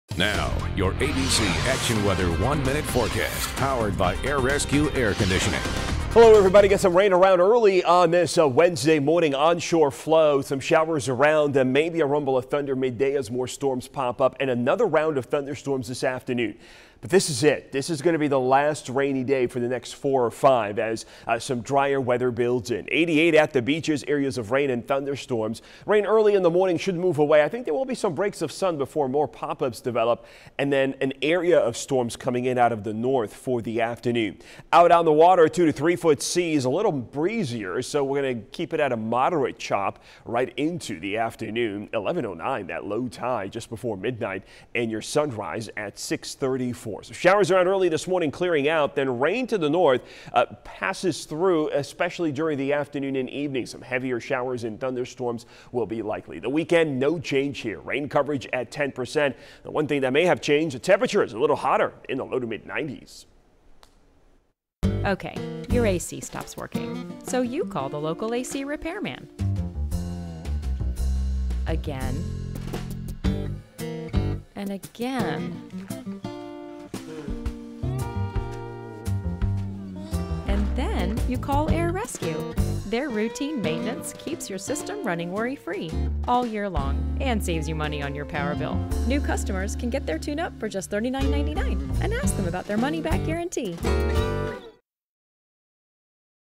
Florida's Most Accurate Forecast with Greg Dee on Wednesday, June 19, 2019
Mostly cloudy, warm and humid. Sct'd areas of rain and storms through the day. Highs in the 80s.